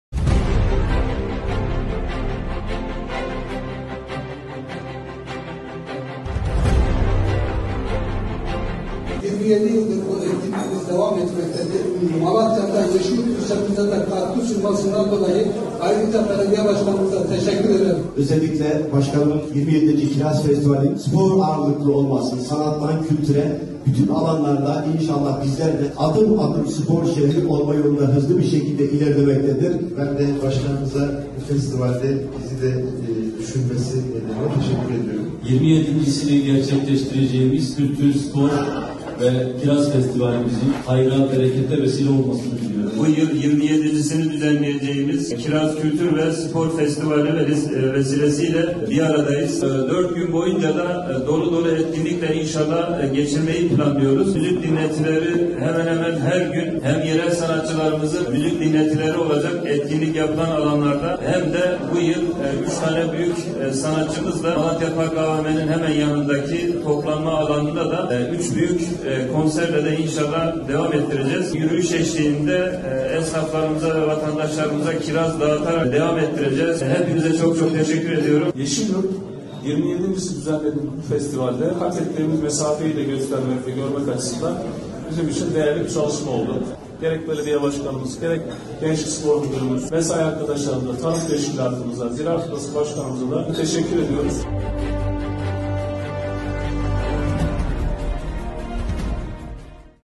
Yeşilyurt Kültür,Spor ve Kiraz Festivali’miz için tüm şehir hazır ✅
Bu yıl 2️⃣7️⃣. olarak düzenlemiş olduğumuz Yeşilyurt Kültür,Spor ve Kiraz Festivali’miz için tüm şehir hazır ✅

23-26 Haziran tarihleri arasında buluşalım 👋

Malatya Yeşilyurt Belediyesi Resmi Youtube Sayfası